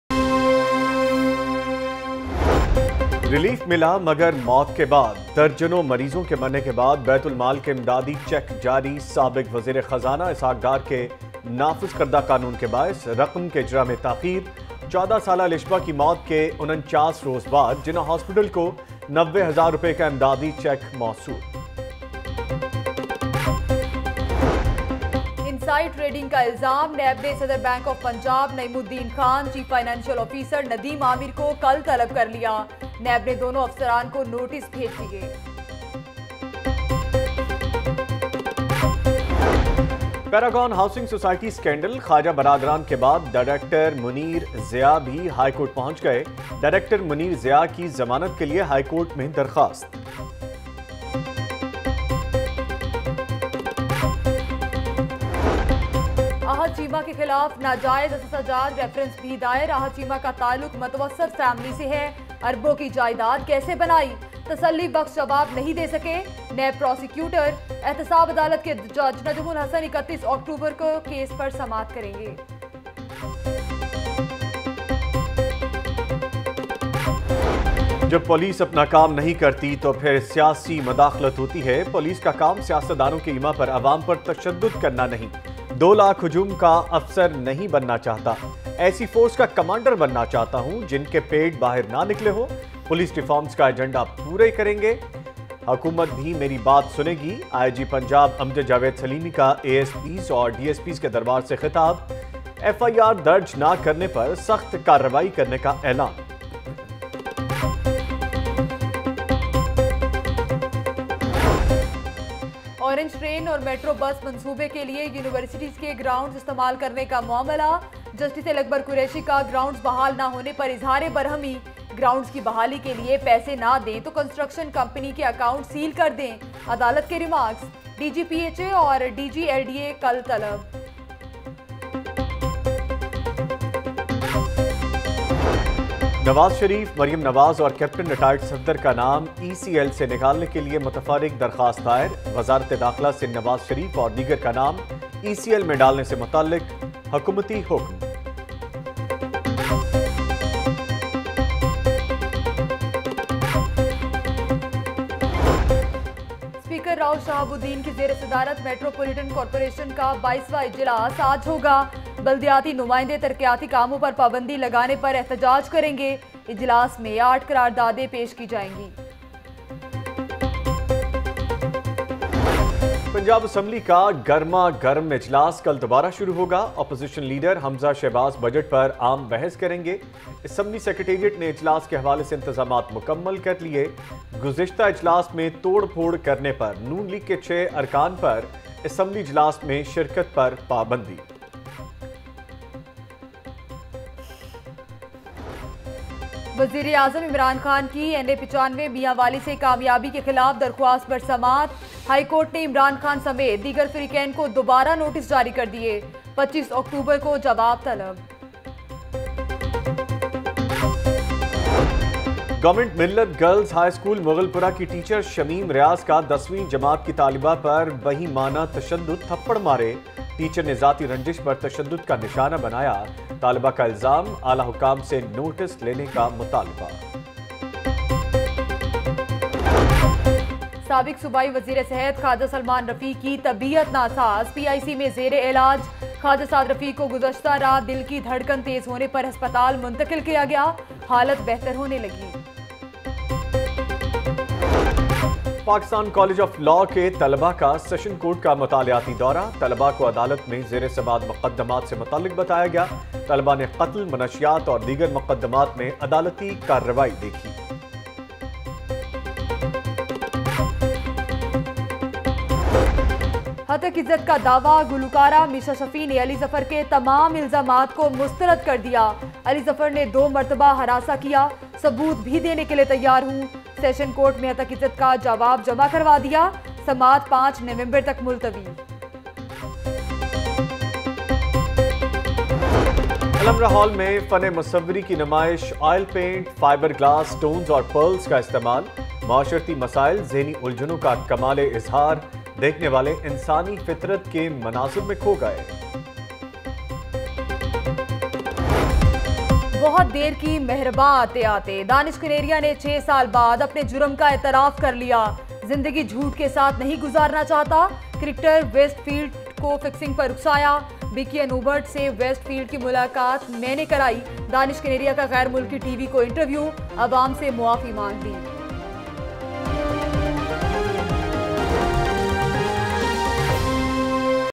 12 PM Headlines Lahore News HD – 18 October 2018
Watch Latest News:
NAB approves 14 days physical remand of Shehbaz Sharif, Saad RAFIQUE appears before NAB in Paragon Scandal, Punjab Assembly to present financial budget today, NAB issues transit remand to Shehbaz Sharif, Hamza Shehbaz media talk outside Punjab Assembly, Rana Sanaullah bashes ruling govt outside NAB Court, PML-N workers protest outside NAB Court, Students block canal road, Immense traffic jam in Lahore, Scuffle breaks out between locals and traffic wardens, Dollar prices witness sharp increase, Oil tanker collides at Waris Meer underpass and much more.